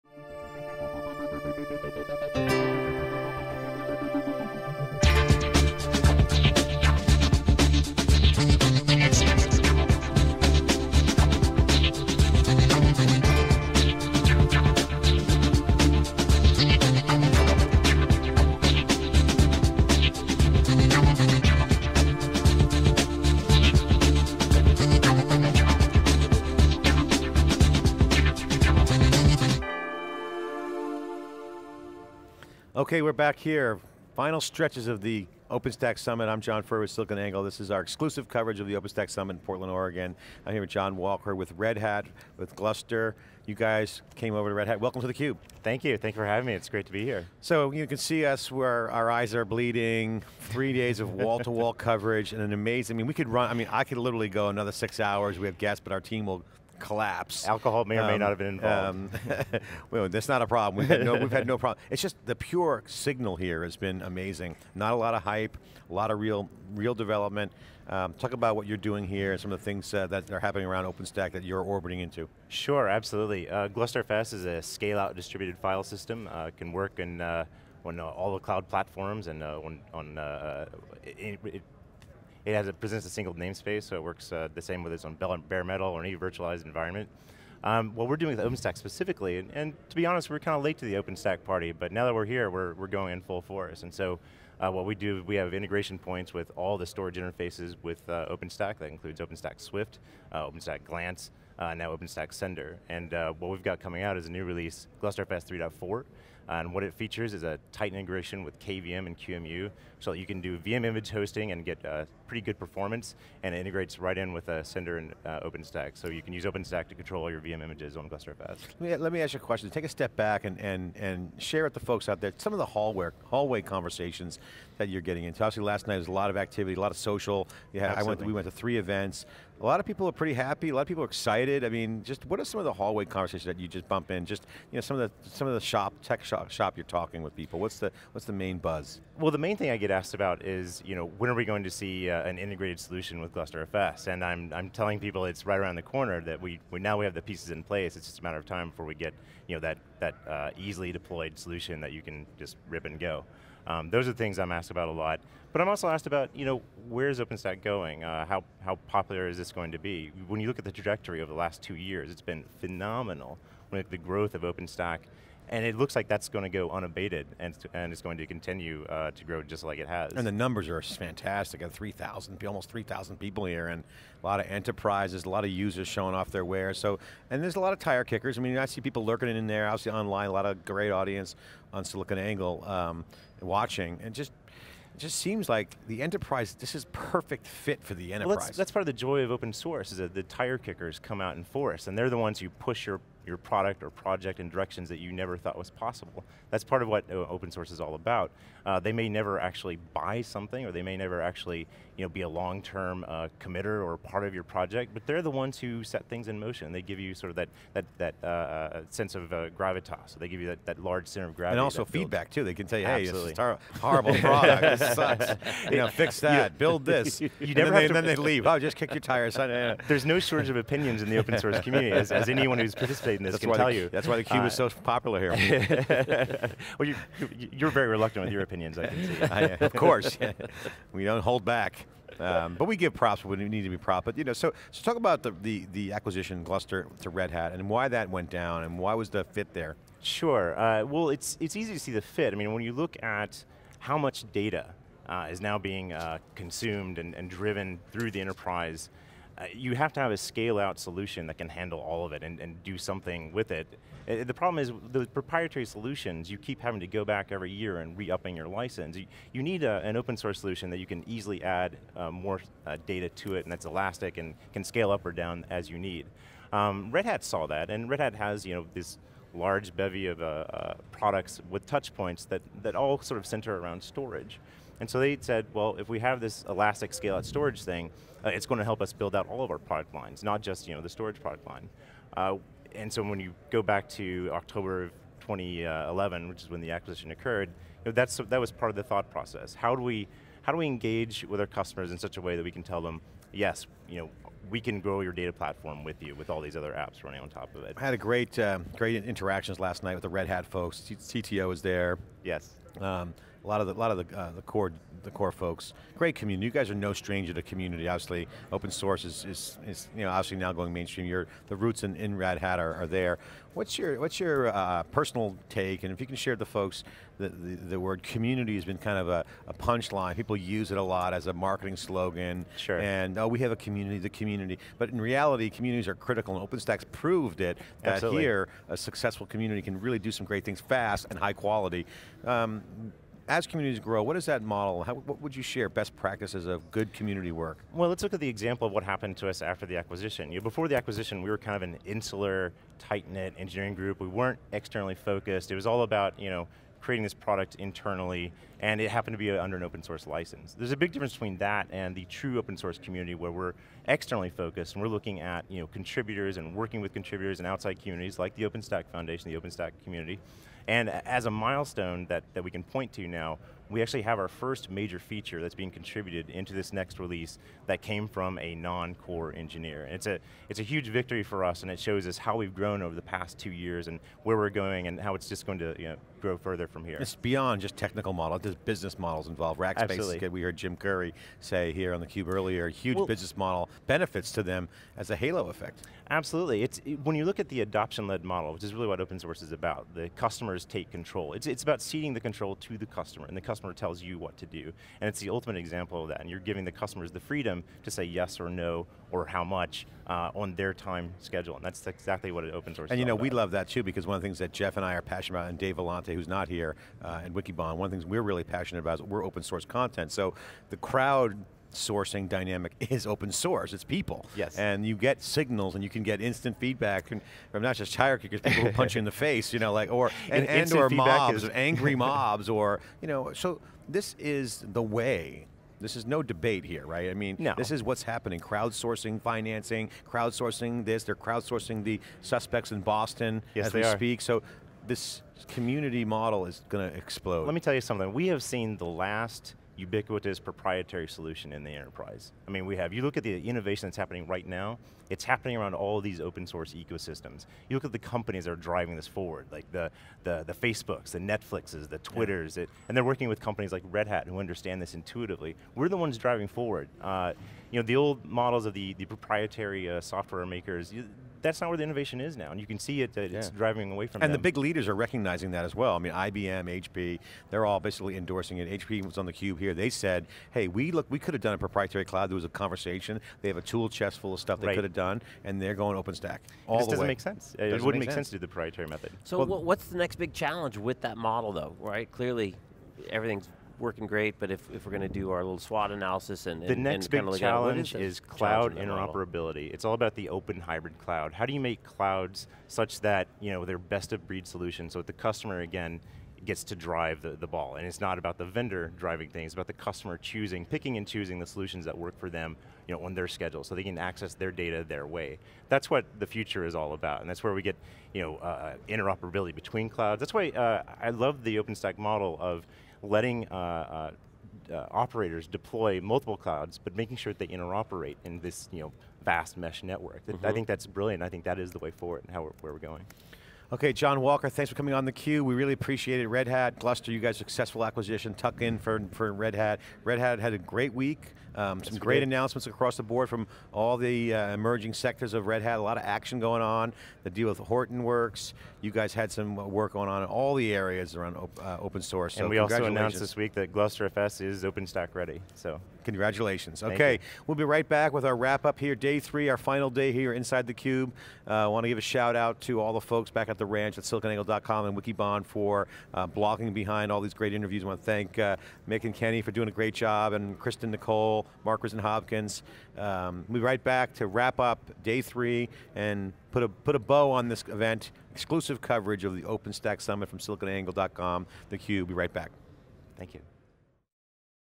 John Mark Walker - OpenStack Summit 2013 - theCUBE
John Mark Walker, Red Hat, at OpenStack Summit with John Furrier and Jeff Frick
Back in 2011, or 200 years ago in Internet time, Red Hat announced an important acquisition when it acquired Gluster, an open source storage software company. John Mark Walker – Gluster Community Lead at Red Hat, stopped by as our last guest for our OpenStack Summit 2013 coverage by theCube. Co-Hosts John Furrier and Jeff Frick gave the floor to Walker to discuss what Gluster and Red Hat saw happening in the community.

Gluster was a perfect acquisition target for Red Hat’s cloud computing strategy two years ago because Gluster is specifically optimized for emerging large-scale content-centric workloads, rather than for use as a general purpose storage technology. As a storage solution, an important feature of Gluster is that it is a pure software solution, able to run on commodity storage hardware.

The Red Hat Gluster community is a very active community. And as you’d expect with an open source community, there is no shortage of opinions. In working with a very active an opinionated community, the guys asked Walker for some best practices of good community work:

    Encourage and help the community commit code (recently had its first major feature that came from a non-core engineer)
    Business models can be created, allow those to foster
    Halo effect is real, be mindful

He believes we’ve seen the end of the proprietary solutions in the enterprise as we know it. “Open source is what is driving things forward,” says Walker.  So what’s the next big challenge in that transition? Cloud interoperability between clouds, not operators. Users getting their data, their way.